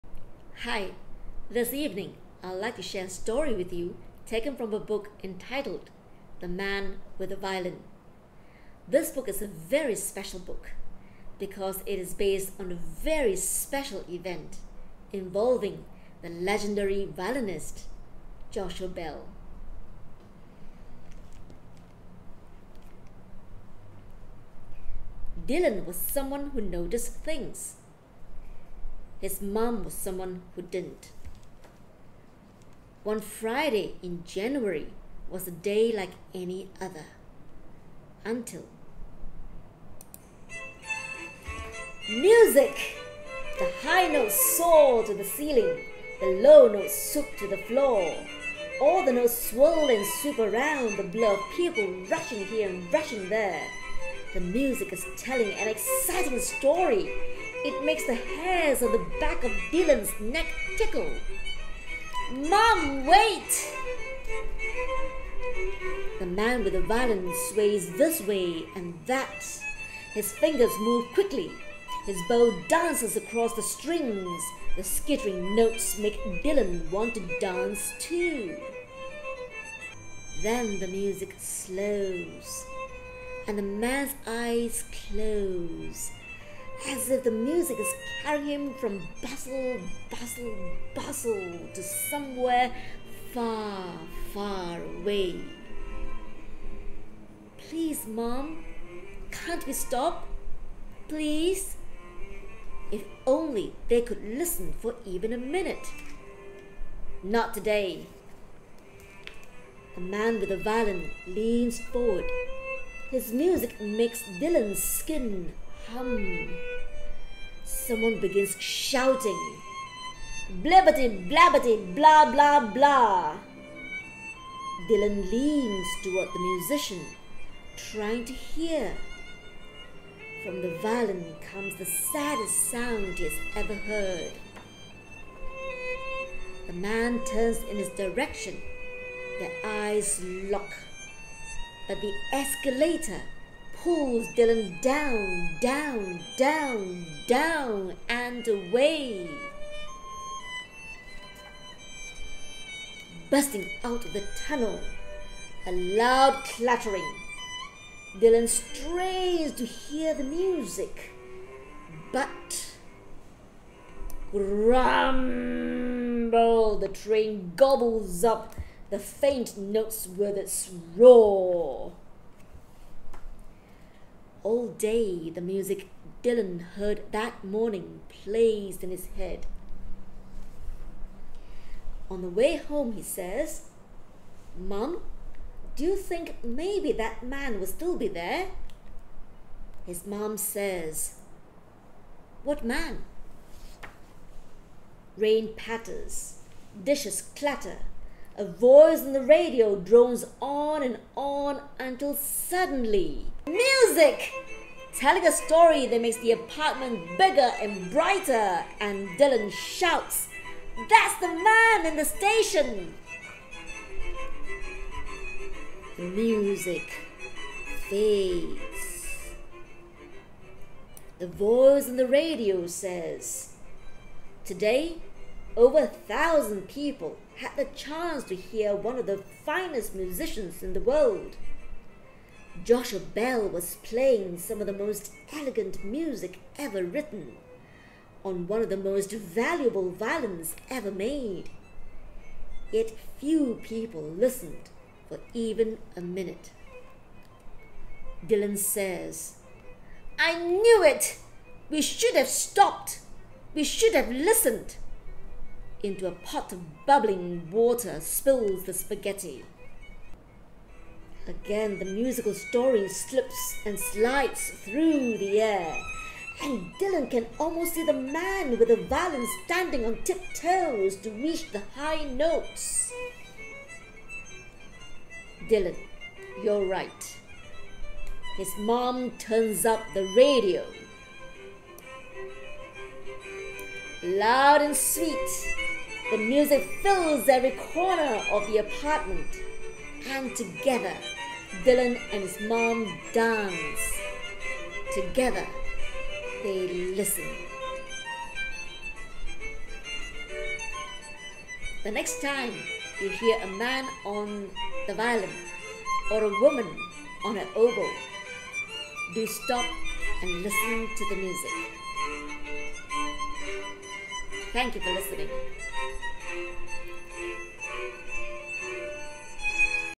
The Man with the Violin
'The Man with the Violin' is written by Kathy Stinson and illustrated by Dusan Petricic.